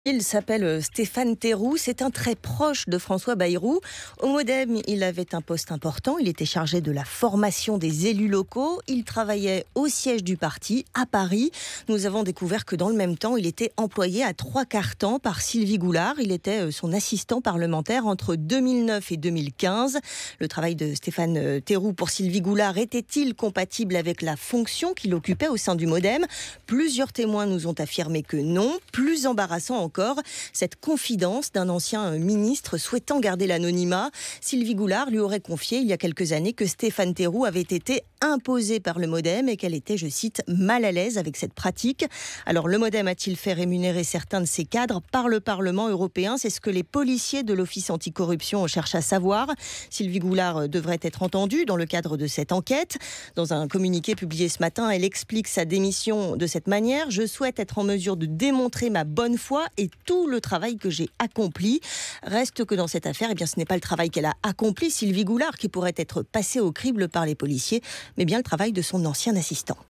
Sylvie Goulard démissionne : précisions
Sylvie Goulard démissionne : précisions à propos de l'attaché parlementaire au cœur de l'enquête préliminaire visant le MoDem